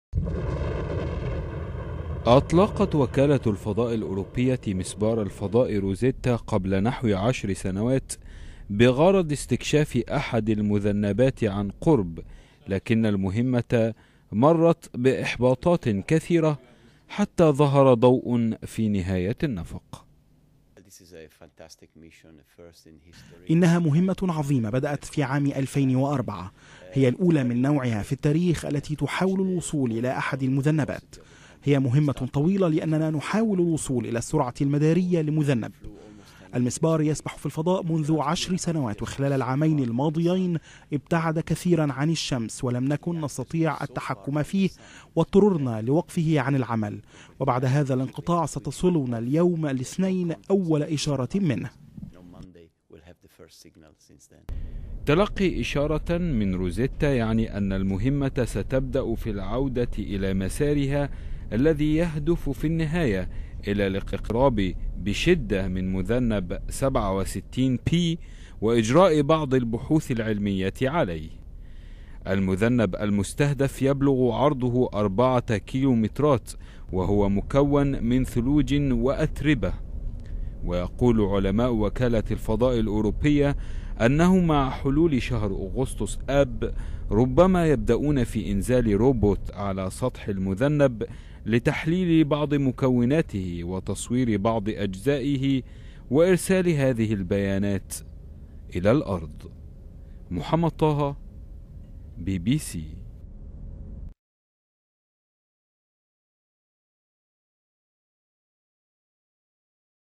عودة مركبة الفضاء روزيتا الي العمل بعد عامين من التوقف
مركبة الفضاء روزيتا تعود إلى العمل بعد عامين من التوقف. وتستعد روزيتا للدوران حول مذنب في مهمة غير مسبوقة بدأت منذ عشرة أعوام.